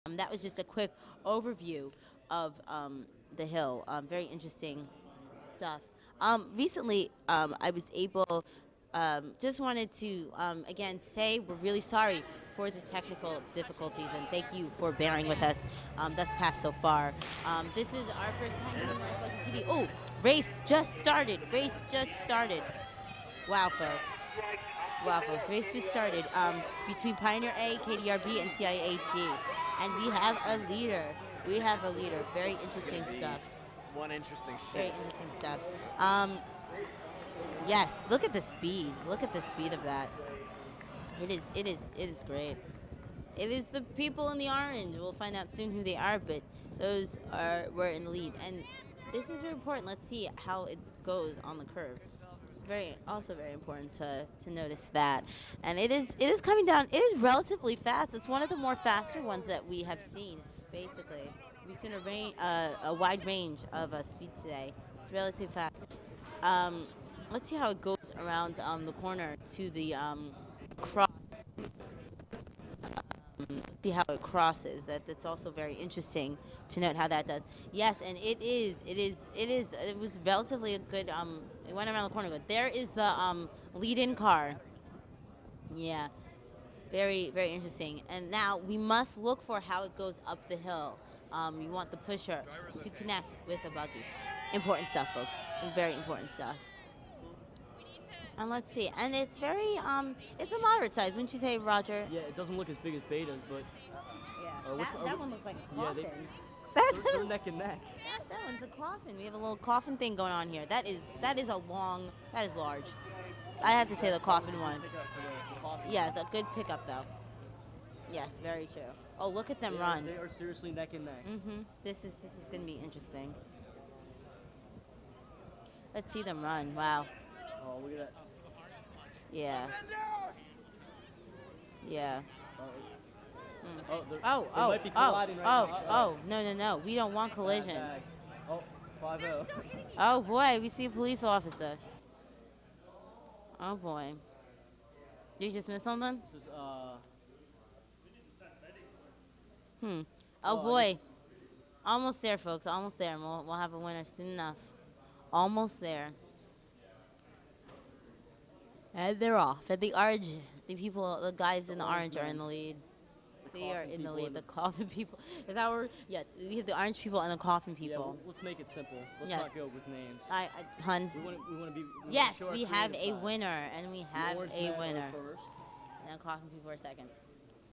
2001 CMU Buggy Raceday - Friday Men's Heat 7 (Pioneers A KDR B CIA D)
2001 Buggy - Friday Men's Heat 7
Lane 1 - Pioneers A (Helios) - DNF
Lane 2 - KDR B (Pagan) - 2: 33.97
Lane 3 - CIA D (Stealth) - 2: 38.00

00:15 - Race

Friday Men's races were rained out after Race 8, so Friday times are unofficial.

Buggy (also called Sweepstakes) is a race held annually since 1920 at Carnegie Mellon University in Pittsburgh, Pennsylvania. Teams of five athletes (the pushers) push an un-motorized vehicle (a buggy) containing a driver (an actual person) around a 0.84 mile course in a relay format where the vehicle serves as the baton. Roughly half of the course is uphill, during which the pushers run and shove the buggy uphill. During the other – downhill – half, drivers are on their own to navigate tight turns at speeds of up to 40mph.

Footage courtesy of cmuTV